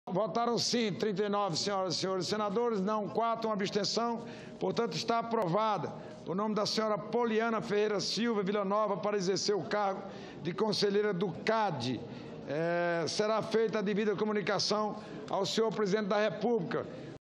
Senado aprova Polyanna Ferreira Silva Vilanova para o cargo de conselheira do Cade
18/10/2017 - Ordem do Dia - Senado aprova Polyanna Ferreira Silva Vilanova para o cargo de conselheira do Cade